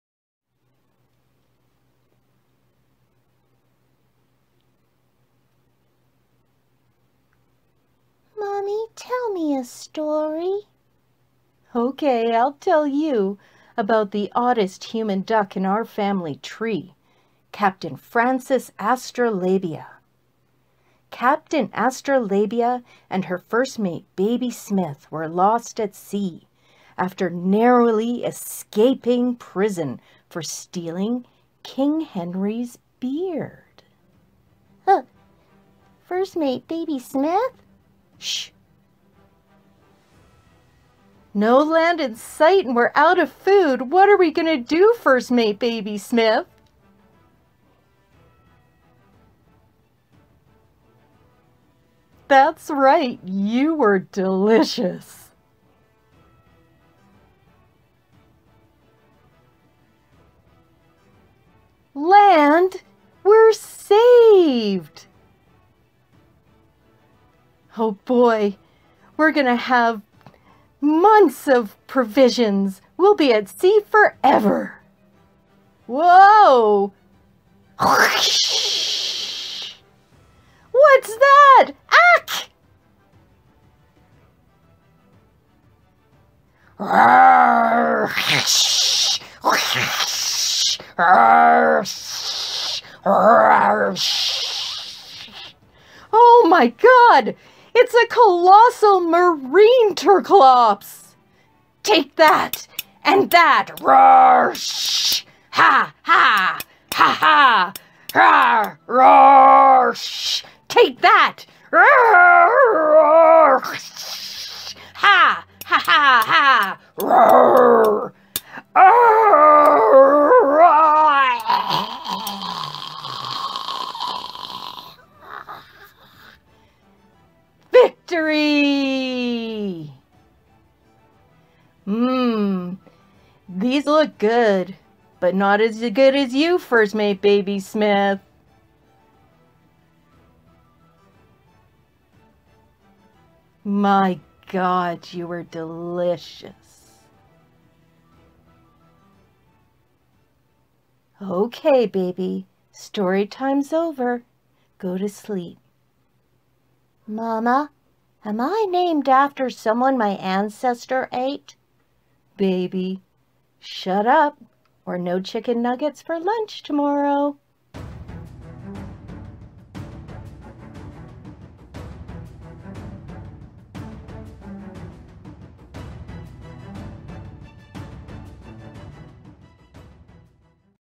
Tastes Like Chicken
Private Investigator and Single Mom Delores Duckingham tells Baby a story about a 15th Century ancestor.
This piece was shown as part of Calgary Animated Objects Society Slamalicious III Puppet Slam. Just learning how to master editing technology. It's been fun...at times...